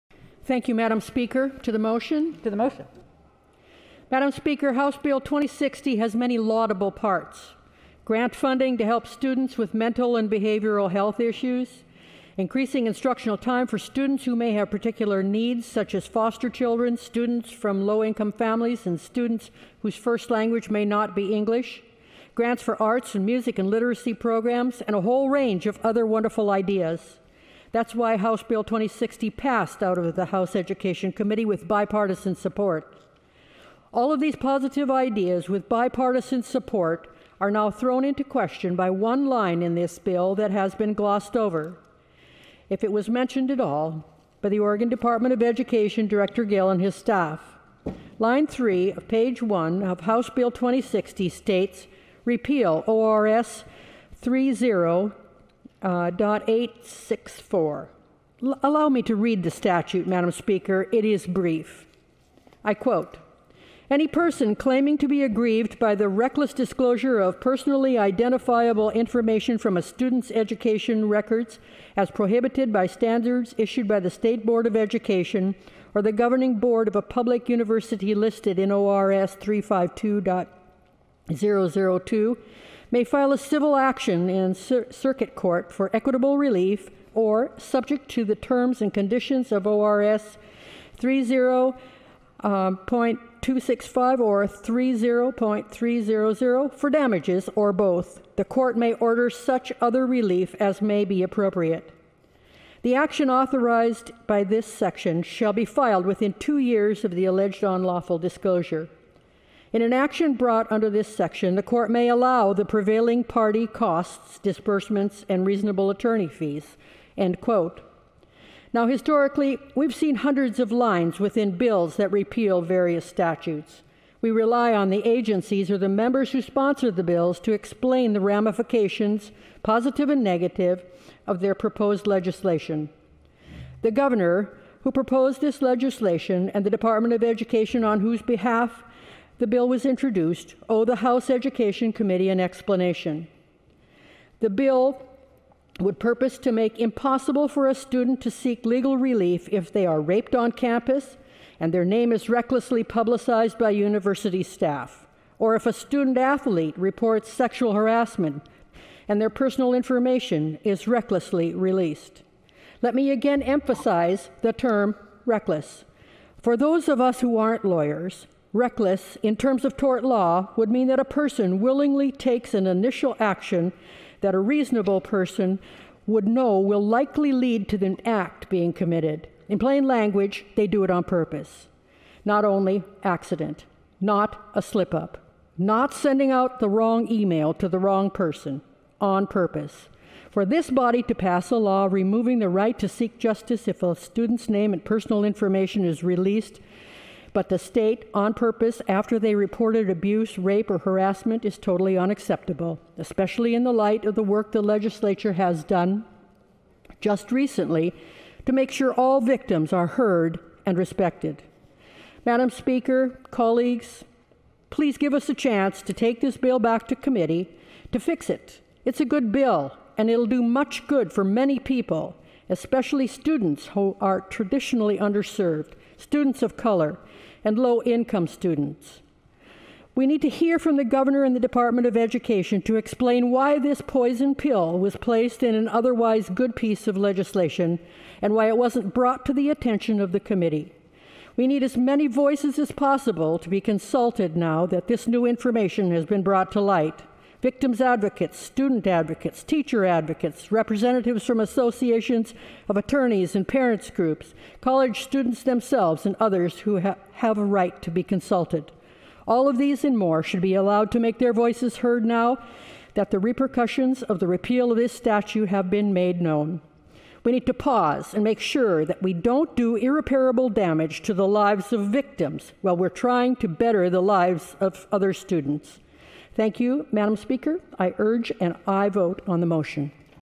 Oregon State Rep Suzanne Weber HB 2060 Floor Speech March 22, 2021
With more than 4000 bills being rushed through this 2021 "Regular" legislative session,  we don't have time to investigate each as thoroughly as should be done.  We need to have time for ourselves as legislators as well as the public to be able to thoughtfully consider the ramifications of each line of every bill that is presented to us.
Fortunately, in this case, we were able to stop HB 2060 from moving further forward when we realized there was one line in this otherwise good bill that would hurt students.
We need to pause and make sure that we don't do irreparable damage to the lives of victims while we are trying to better the lives of other students.